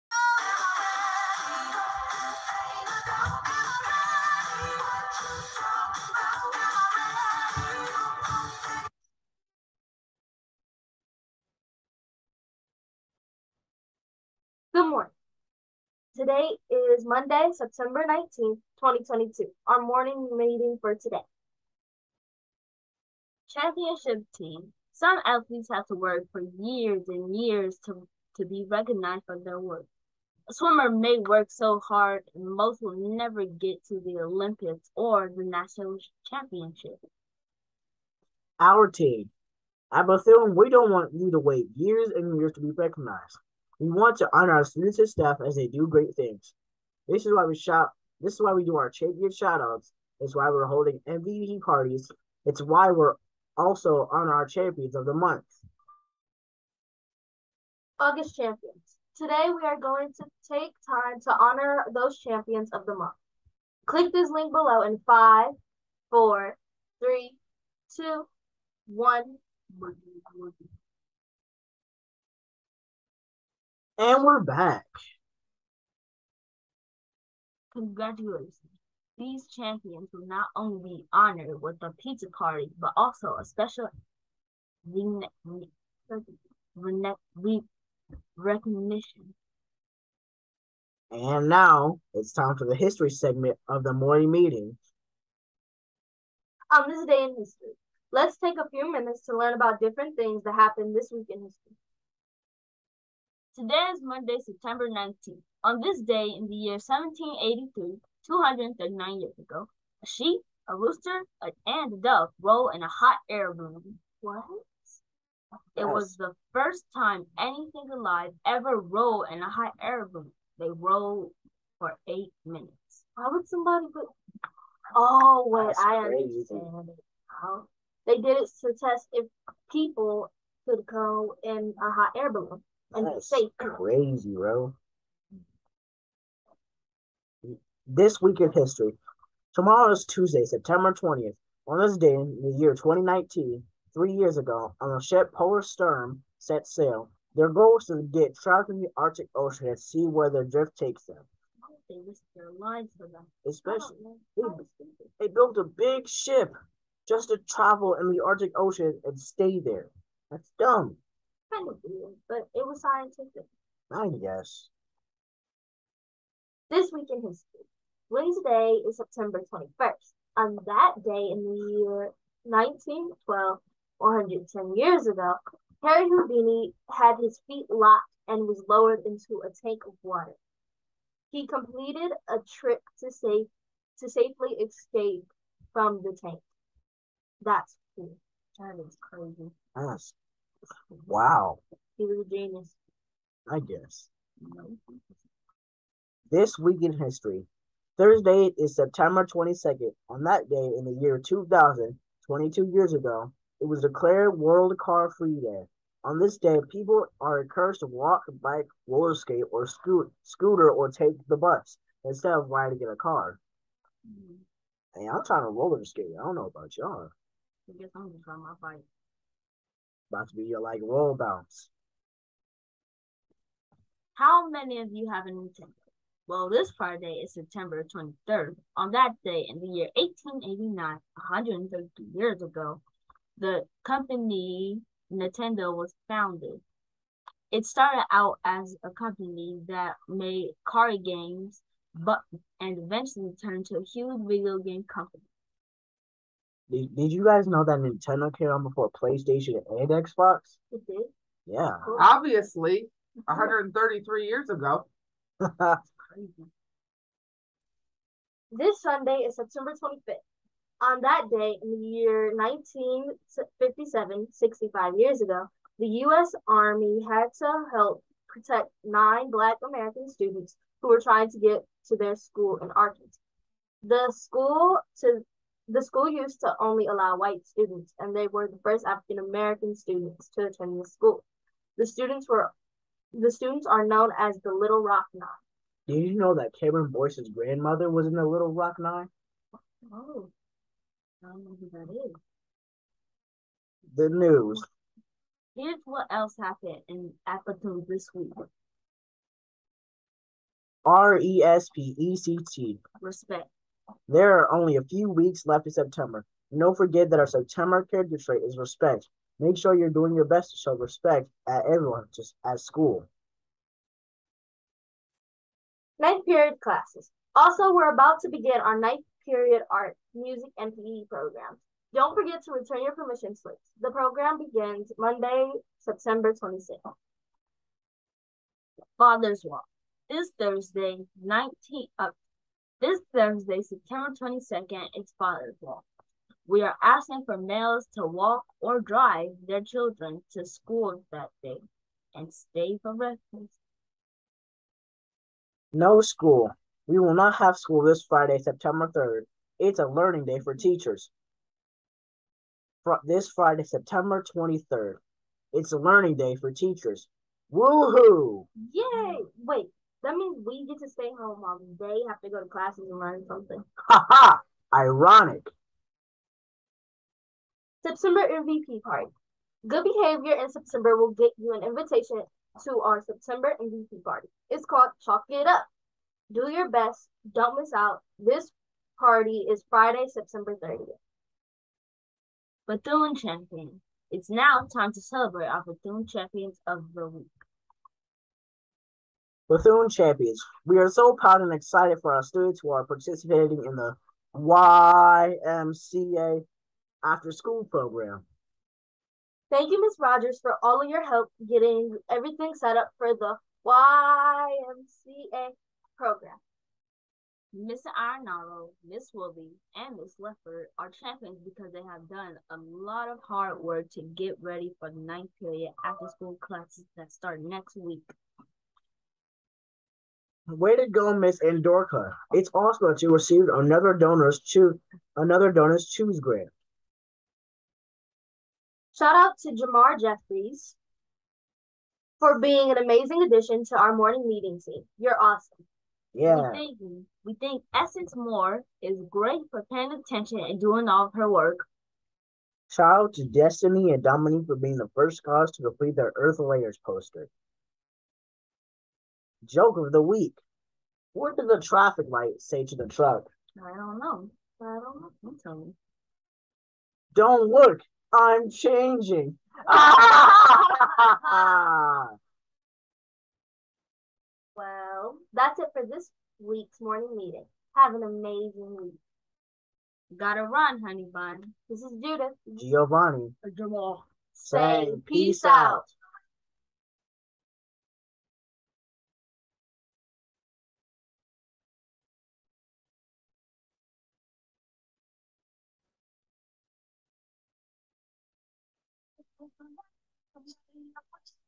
Mary M Bethune PK-8 Morning Meeting 9.19.22
Let's learn about what's happening this week at Mary M. Bethune!  Also, come along as we honor our school's August, Champions of the Month!